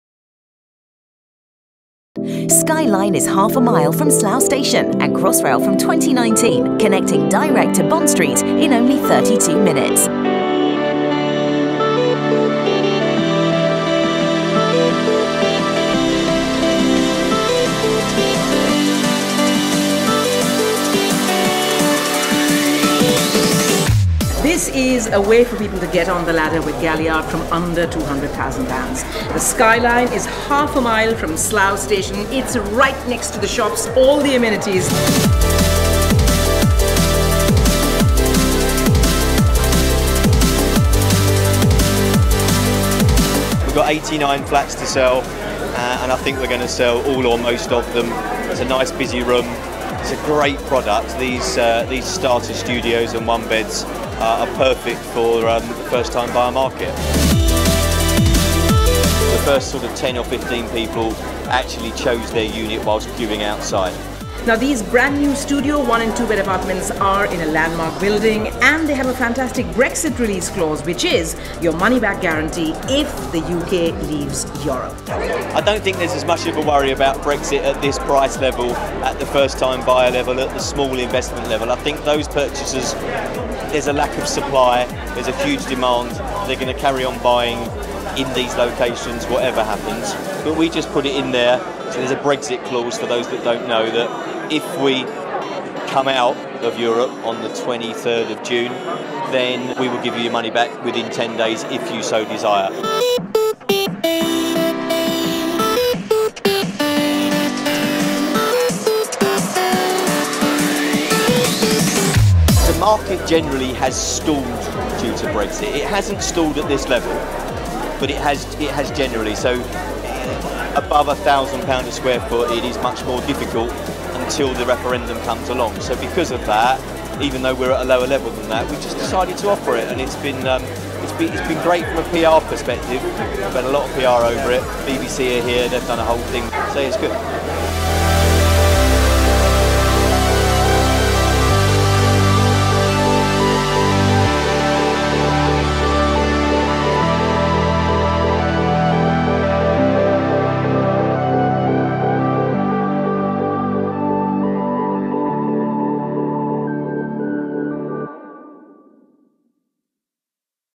The Launch of Skyline, Slough | Galliard Homes
Relive the sales launch of Skyline in Slough, which took place on 9th June 2016. Hear from Galliard's Sales Director, David Galman, who talked about the rising residential popularity of Slough, and have a sneak peak at the exquisite CGI visuals of the apartments.

With prices having started from just £199,995, first-time buyers flocked to the launch for the opportunity to climb the property ladder in a fantastic location within close reach of the capital. Buy-to-Let landlords were also amongst the crowd, hoping to bag an affordable investment opportunity to add to their property portfolio.

Skyline – formerly an office building for information technology equipment company, Fujitsu – will be a striking conversion, housing 89 contemporary studios, one and two bedroom apartments, all purposely built to exude style and offer the utmost in comfort and convenience.

With Crossrail expected in 2018/2019 at Slough station, investors and homeowners can expect to see an impressive amount of capital growth on their property, especially since Skyline will be just half a mile away. Once services commence, residents commuting from Slough can be in Bond Street in the West End in just 32 minutes.

In light of the EU Referendum, Galliard introduced a new Brexit clause with all reservations taken at Skyline, offering a money-back guarantee if Britain decides to leave the EU.

For more information about the development and the Brexit clause, please